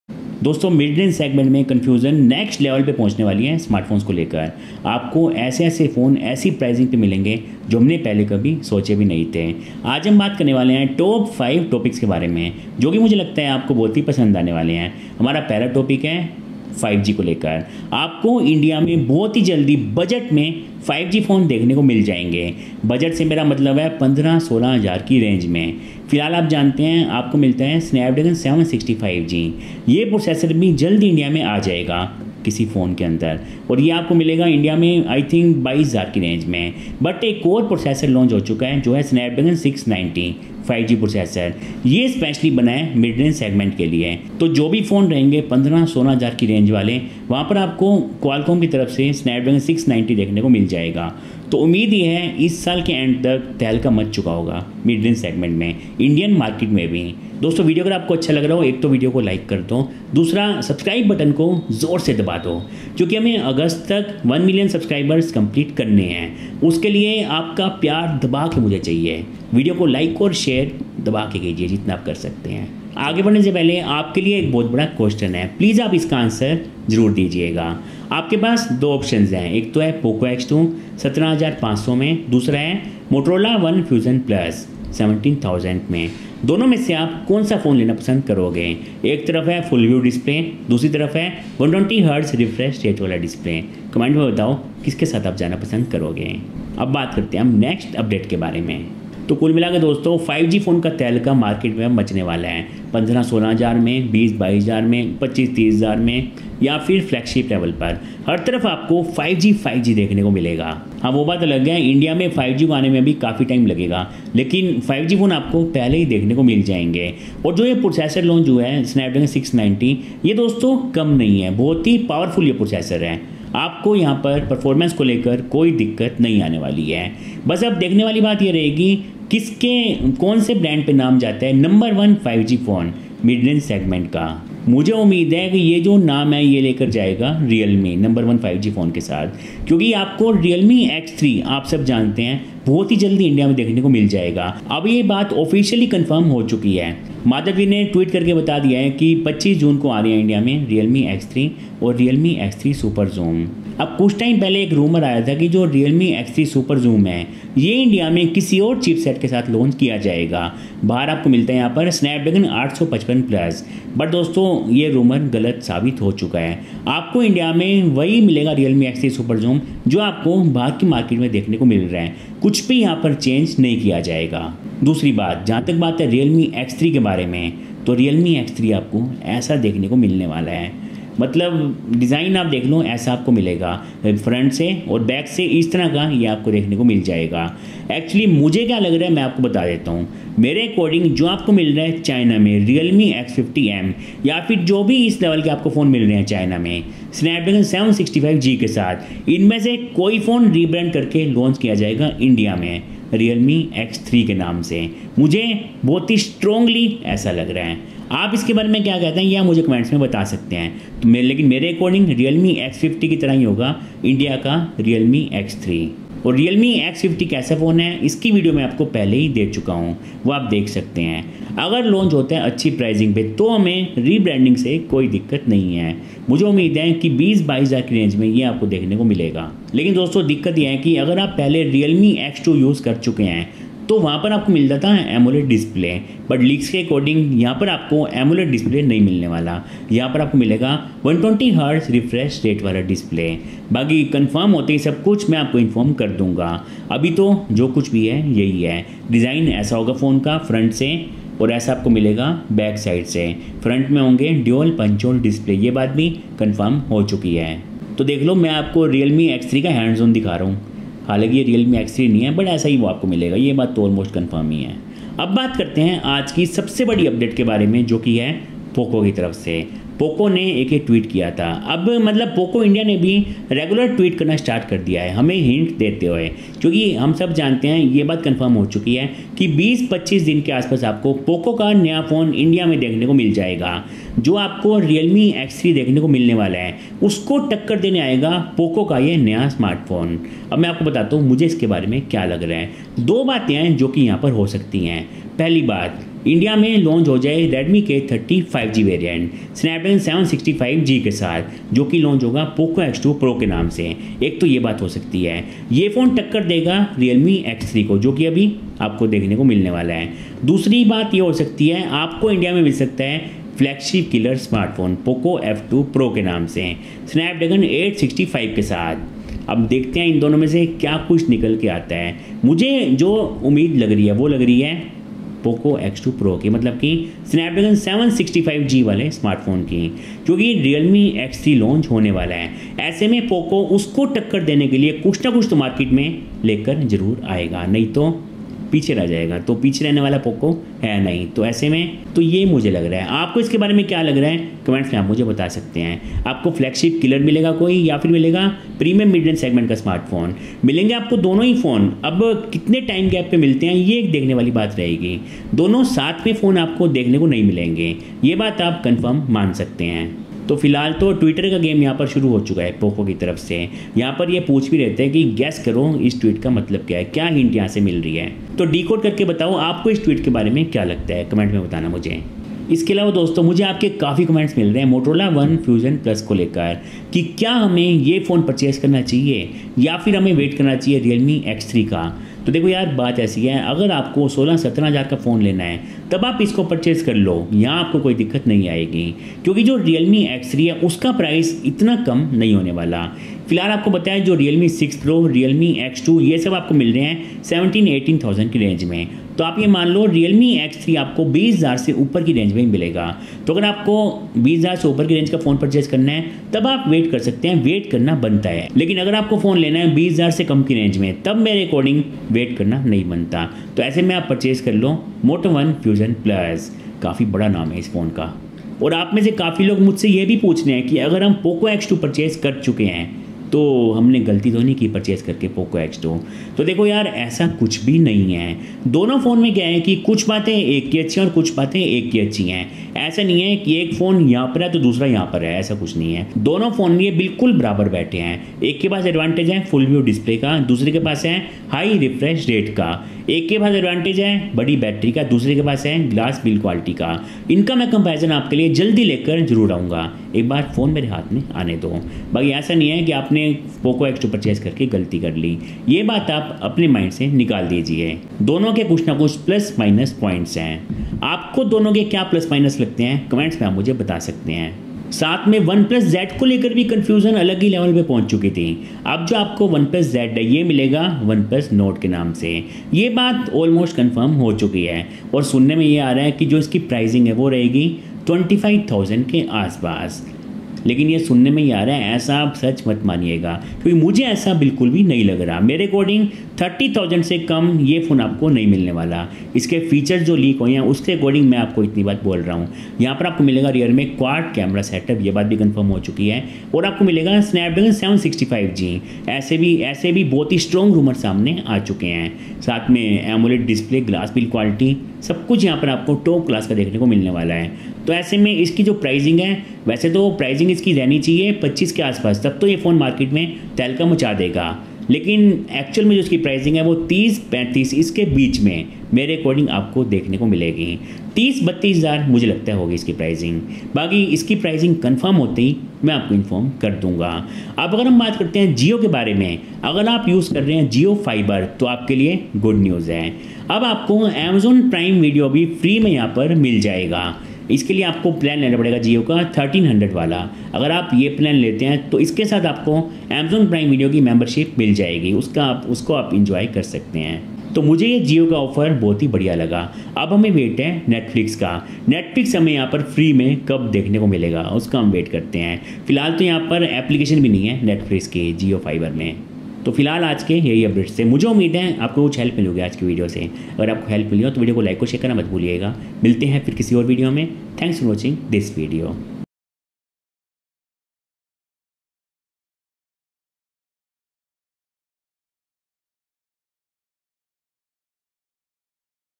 Realme X3 First Impressions? | 14,999-5G Smartphones in India | Poco Phone With SD-765G?
For some motivation, do watch my Journey from being a TELEPHONE OPERATOR to TECH YOUTUBER :-)

My Journey...